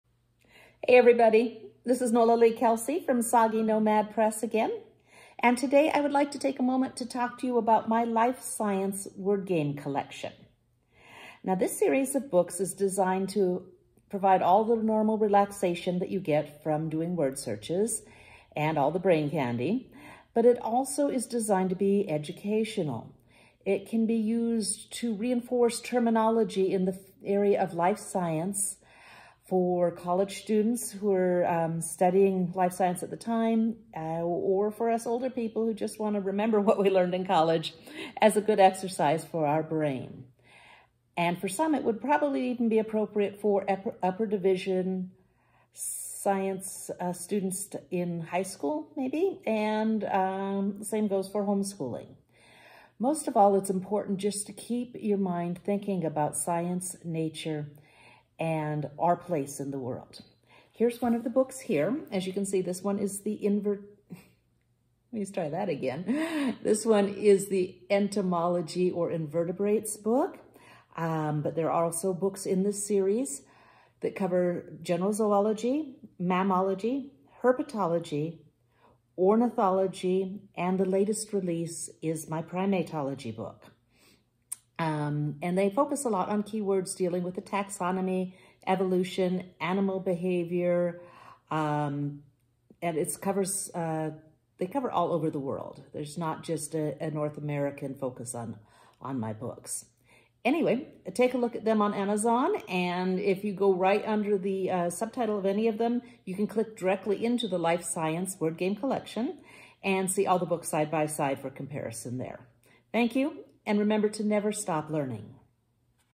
Introduction to the Life Science Word Games Collection of Books for the Naturalist's Soul.
All Books in The Life Science Word Game Collection were curated by zoologist Nola Lee Kelsey and published by Soggy Nomad Press. No puzzles were created from generic online word lists.

Teach, reinforce and recall terminology from biology lessons in taxonomy, evolution, animal behavior and beyond!

Books in this fun, educational series includes:

Zoology Word Searches & Games for the Naturalist’s Soul
Mammalogy: 101 Word Search Puzzle’s for the Naturalist’s Soul
Invertebrates: 92 Word Search Puzzles for the Naturalists Soul!
Ornithology: 112 Word Search Puzzles for the Bird Lover’s Soul

Primatology: Word Search Puzzles for the Naturalist’s Soul

Marine Biology: 96 Word Search Puzzles for the Ocean Lover’s Soul
Herpetology: Word Search Puzzles for the Naturalist’s Soul